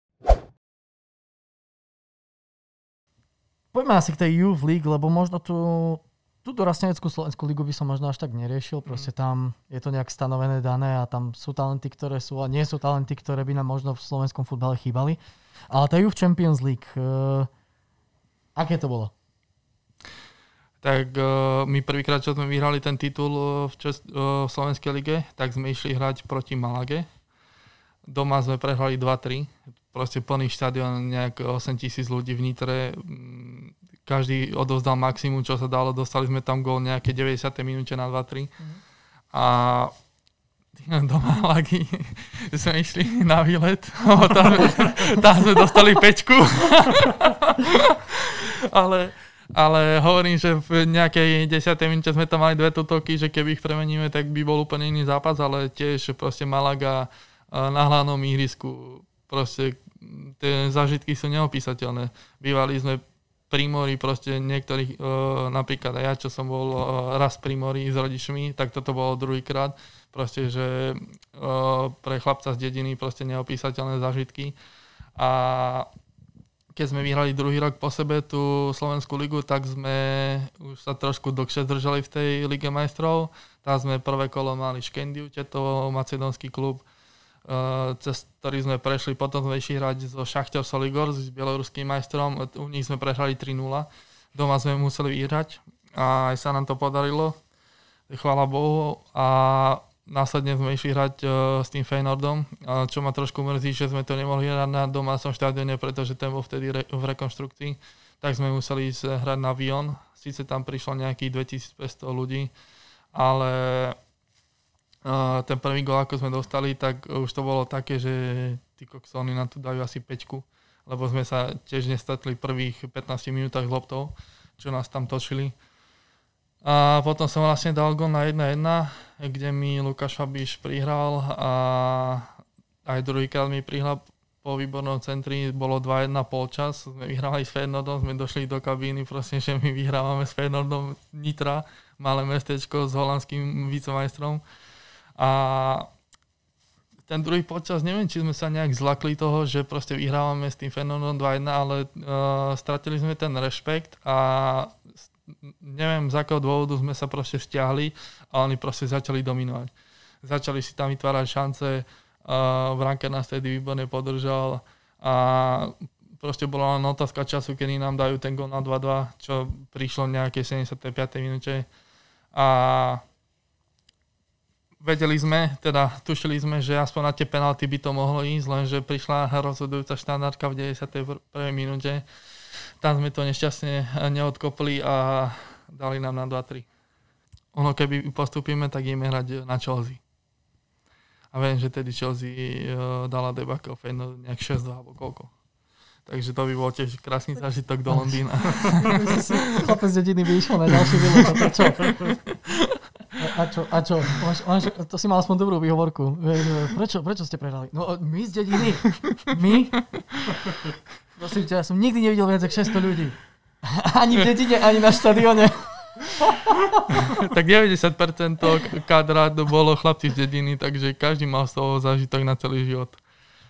Marek Fábry - Výlet do Malagy a zápas s Feyenoordom v Lige Majstrov ∣ KickOff Focus
Marek Fábry, hráč pražskej Dukly, nám porozprával o zážitkoch z mládežníckej Ligy Majstrov ešte v drese FC Nitra.
Vypočujte si ako si chlapci užili Malagu či zápas s obrovským favoritom Feyenoordom.
Celý diel nášho rozhovoru s Marekom Fábrym bude dostupný už v pondelok! :)